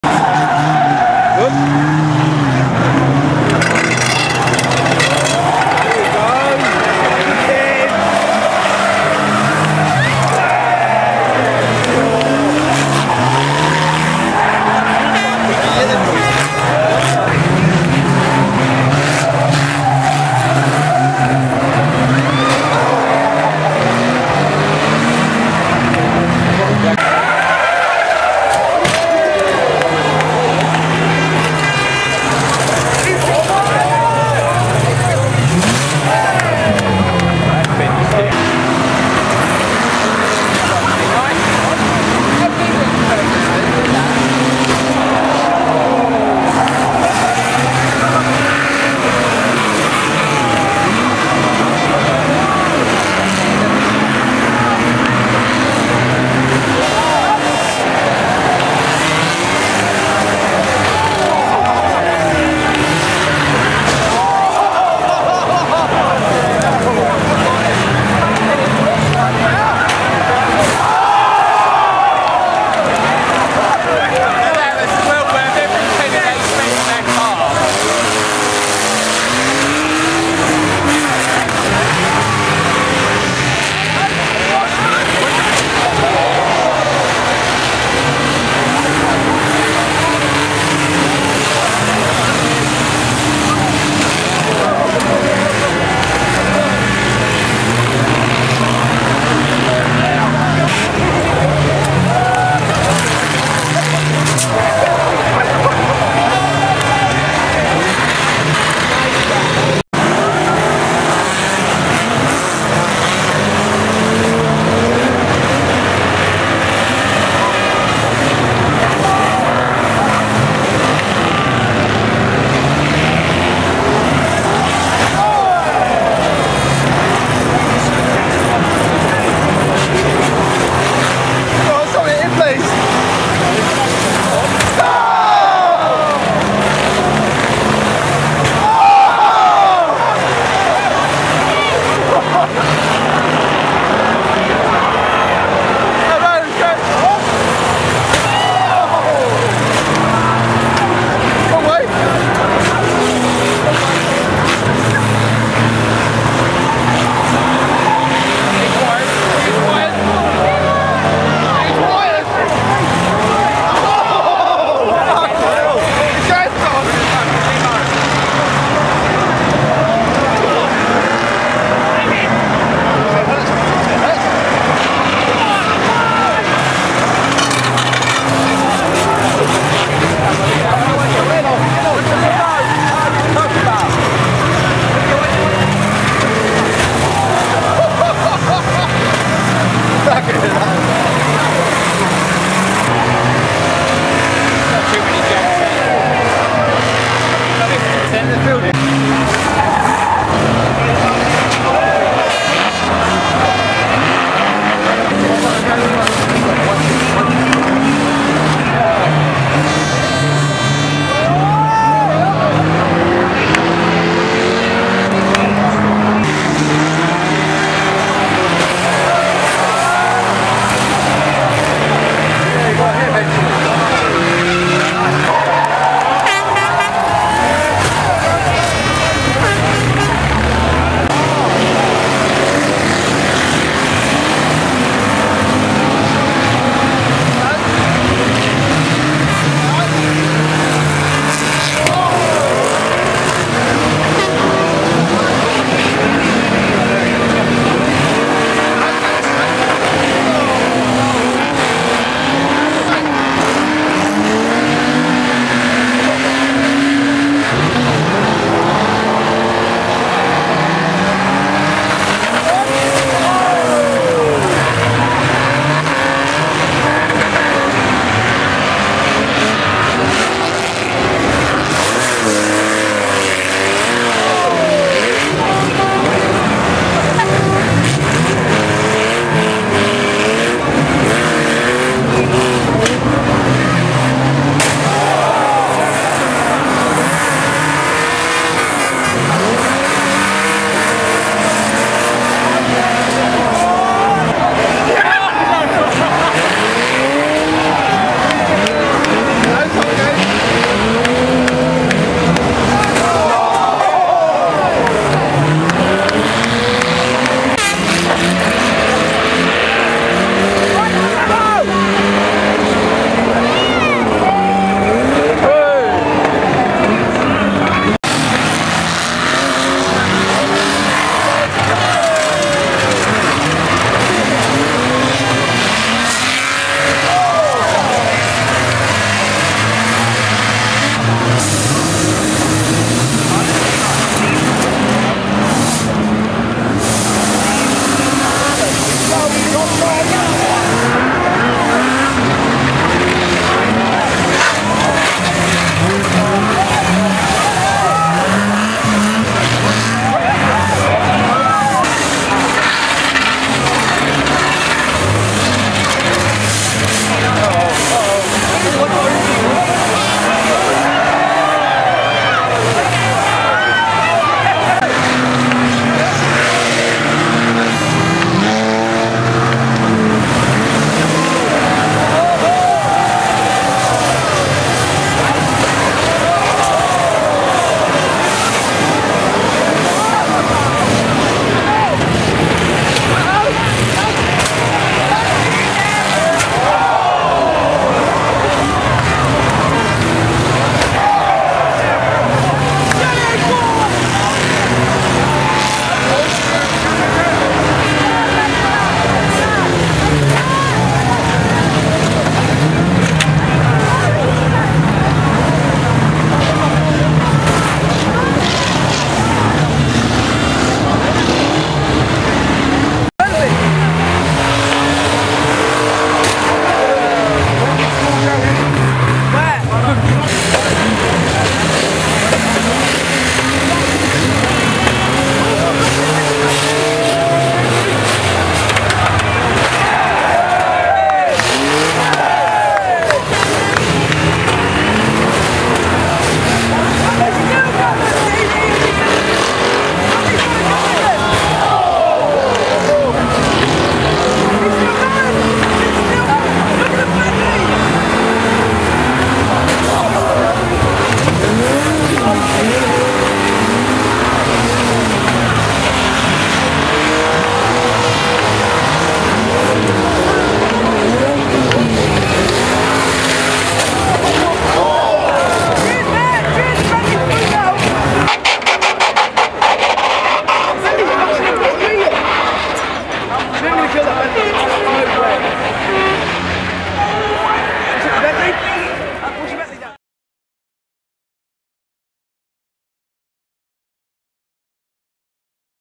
THE FIRECRACKER 07 UK BANGER RACING
ACTION FROM THIS YEARS FIRECRACKER. ONLY 130 CARS THIS YEAR WITCH IS A DROP OF FROM YEARS GONE BY WHEN 200 CARS WAS THE NORMAL TURN OUT BUT THE RACING WAS STILL GOOD AND THE CARS WERE RARE A LOOKED GREAT BUT MOST OF THE ACTION WAS AT THE OTHER END OF THE TRACK IN THE LATER HEATS BUT SOME GOOD HITS WENT IN ON THE PIT BEND WHERE I WAS FILMING. I DID'NT GET ALL THE BIG HITS ON HEARE BUT HOPE YOU STILL ENJOY THIS VIDEO